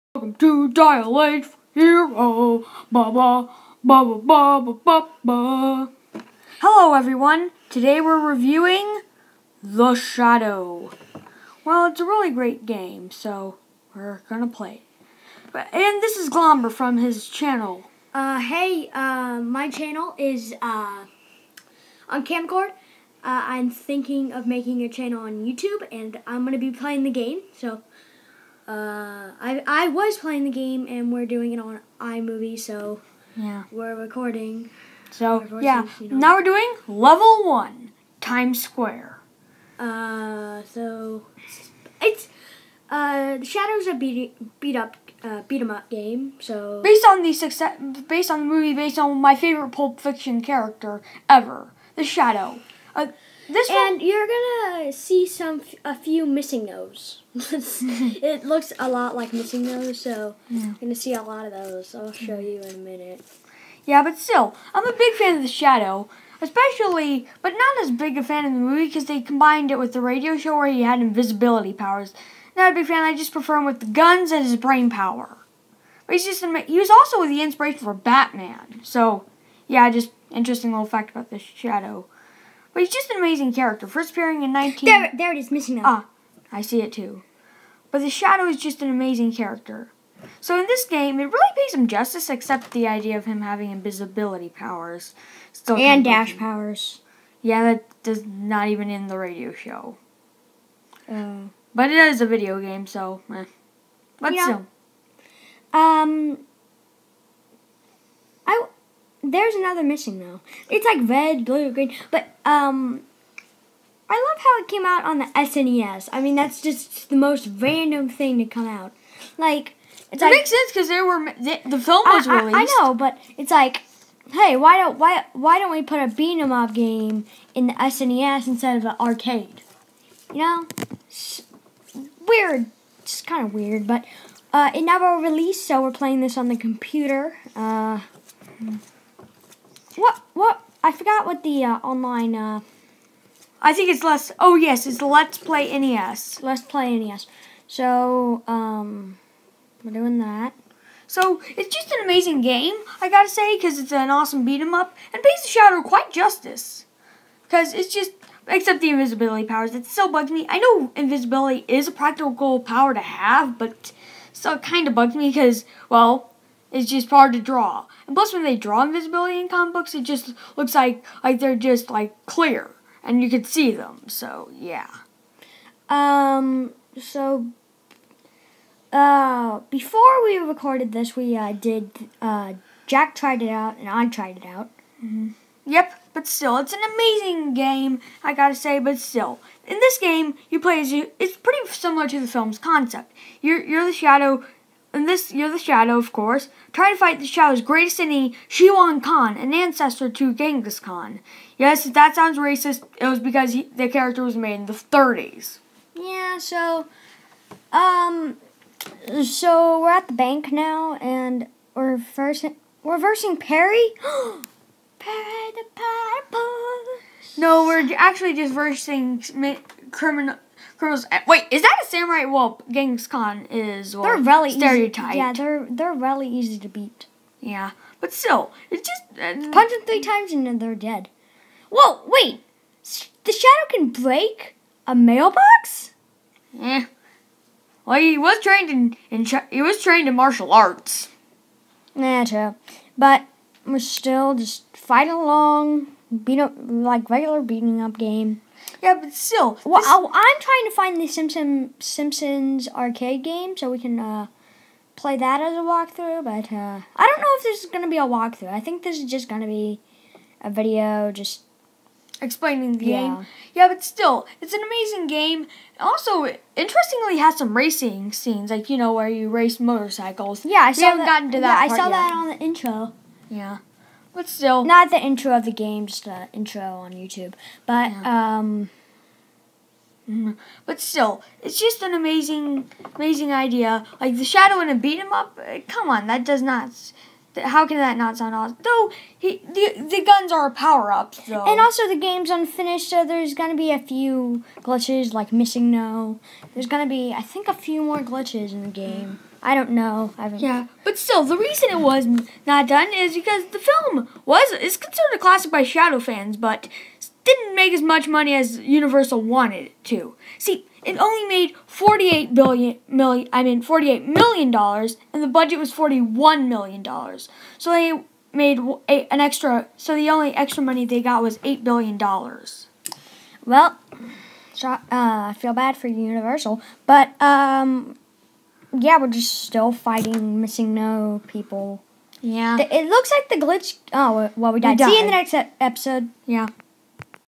The Shadow SNES Game ( ft. Glomber)
Comic ghost and Glomber review The shadow  SNES game!